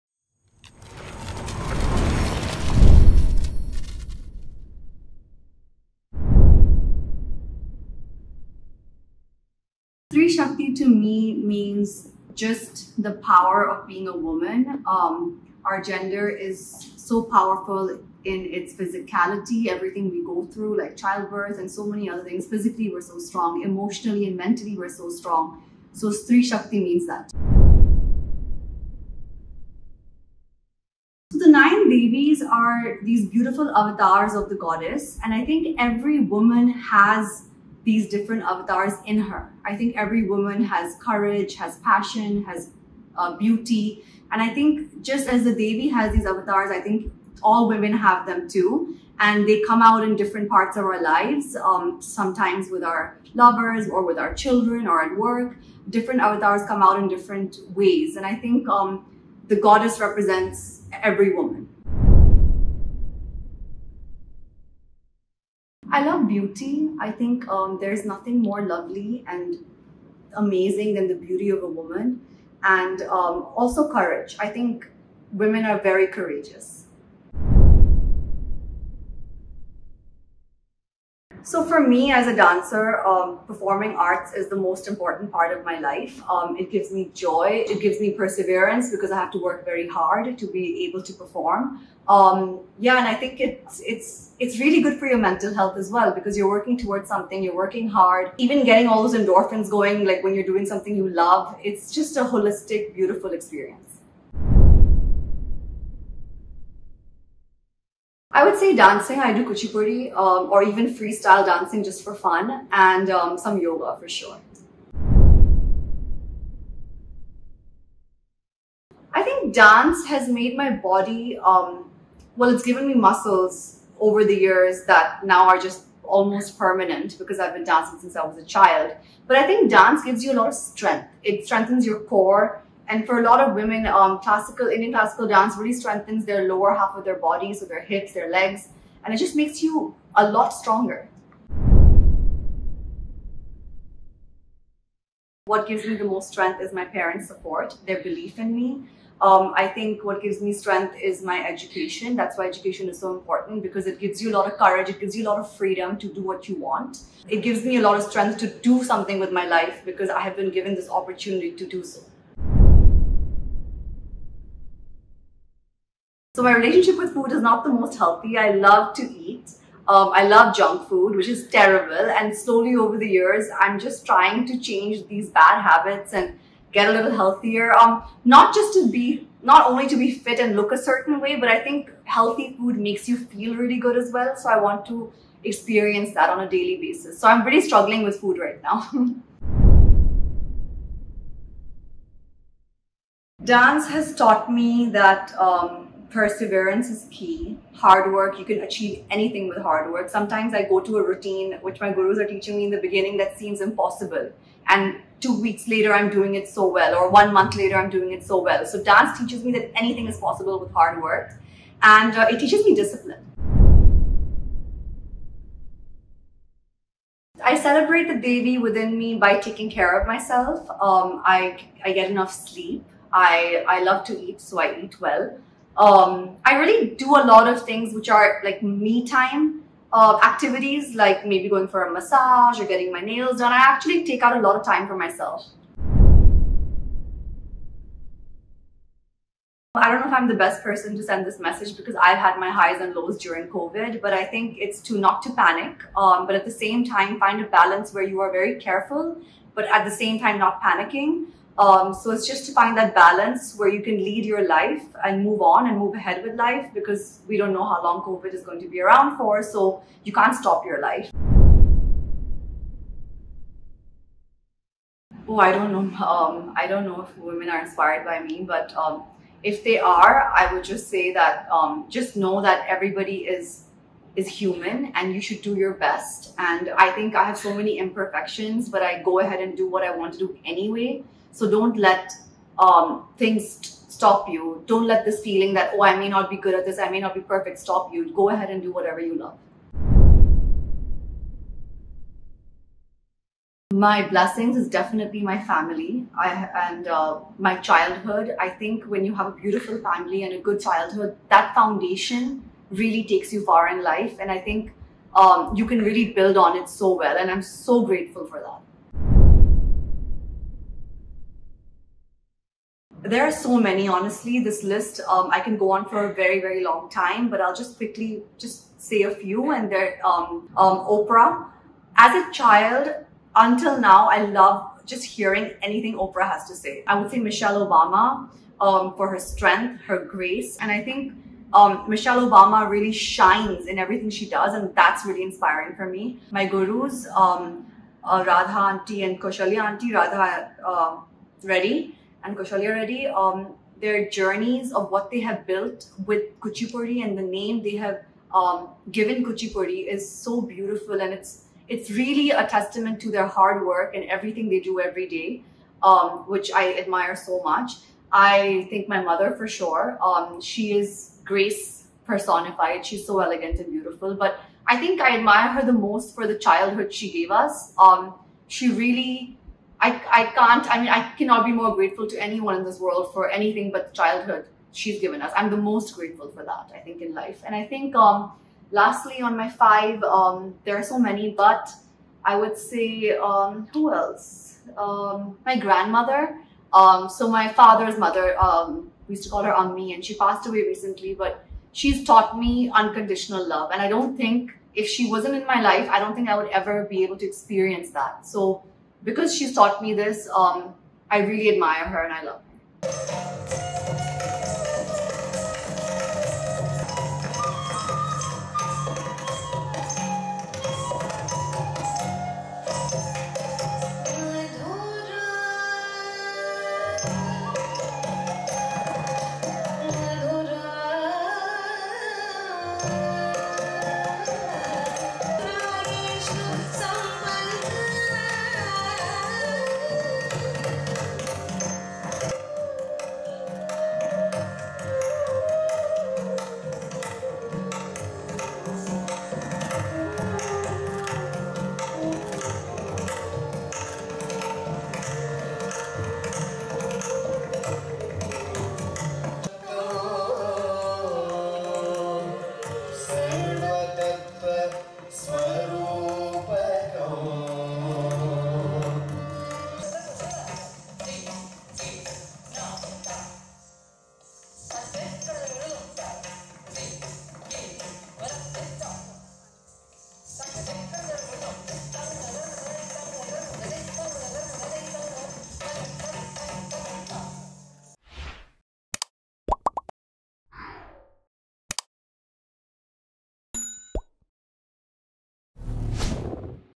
Celebrate the Devi within U with the classical dancer Pernia Qureshi | Upasana Kamineni Konidela
The powerful Entrepreneur and classical dancer Pernia Qureshi shares how she celebrates and channels the Devi within in her!